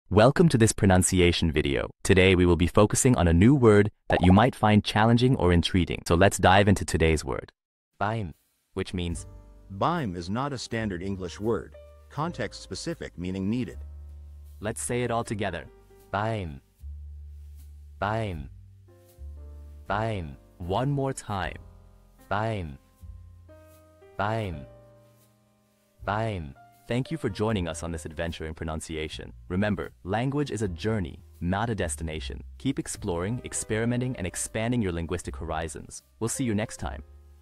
How to pronounce Beim
Master the Pronunciation of 'Beim which means at' - which means : At is not a standard English word;Context-Specific Meaning Needed. 📝 with @PronunciationGerman 🎓🗣️ - [Your Guide to German]

Learn to pronounce 'Beim' perfectly in German with our easy and engaging tutorial by @PronunciationGerman! 📖📽️ In this comprehensive video, we provide you with native speaker examples 🗣️👂 and phonetic breakdowns 🎵 to ensure you get the pronunciation just right.

How to say At the in German